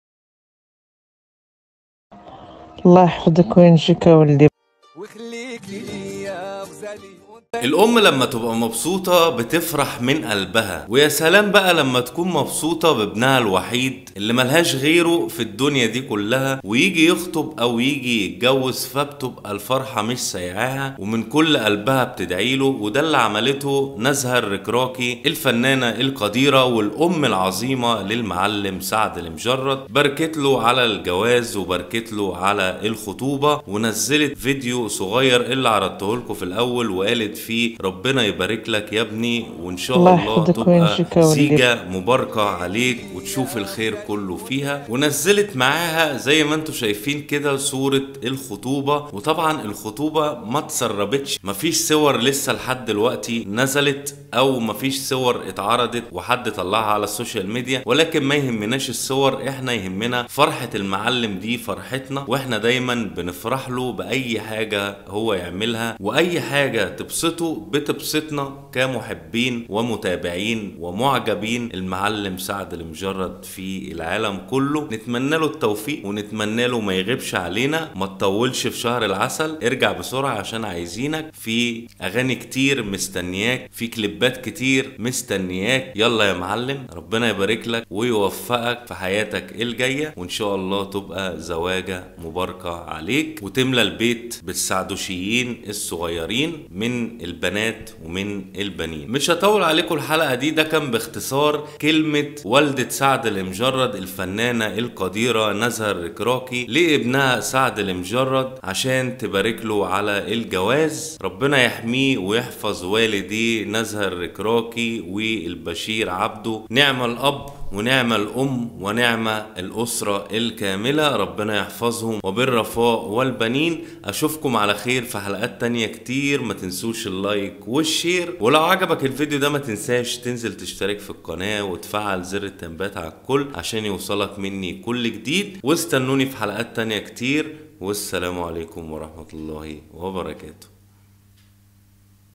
حصريا - نزهه الركراكي تعرض صور خطوبة سعد لمجرد وغيثة .. وتبارك لهم الزواج
منذ اسبوع تم زواج سعد لمجرد وغيثة العلاكي وكان الموضوع مفجأه سعيدة بالنسبة ل جمهور سعد لمجرد في العالم كله و فانز سعد لمجرد .. وفي هذا الفيديو اعرض لكم كيف تبارك الام لإبنها وهي نزهه الركراكي والدة سعد لمجرد تبارك لهما الزواج بعبارت لطيفة ودعوة جميلة من ام لابنها وتعرض احدث صور خطوبة سعد لمجرد وغيثة .

◀ البرامج المستخدمة المونتاج 📸 FILMORA
● برنامج ( Adobe Photoshop 2021 ) في الصورة المصغرة
ــــــــــــــــــــــــــــــــــــــــــــــــ